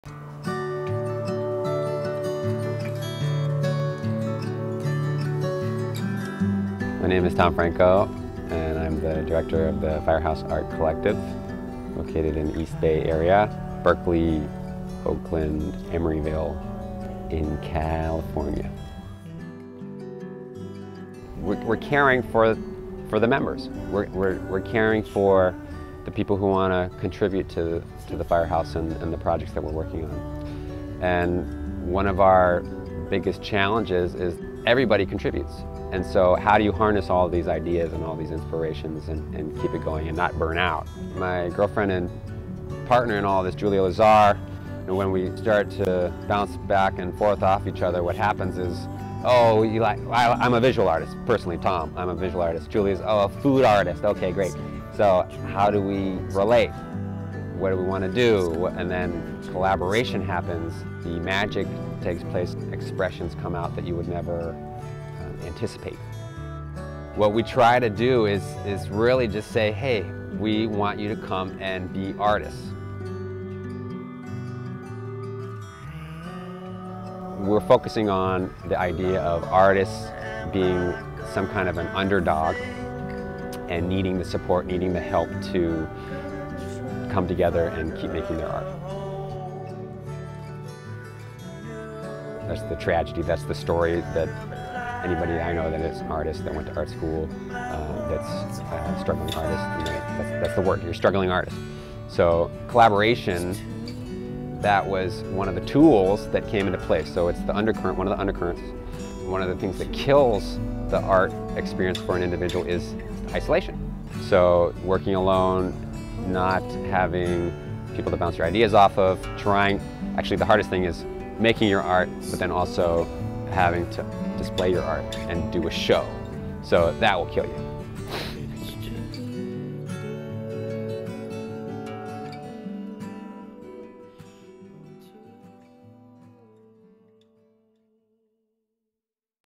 Tom Franco’s Berkeley's Firehouse Bazaar Promo
Tom Franco, younger brother of the actor James Franco, is an artist & founder of the Firehouse Art Collective in Berkeley, CA.

Directed, Filmed & Edited by Joe W. Shapiro

Additional Camera Work by Steven Chillous

joe w. shapiro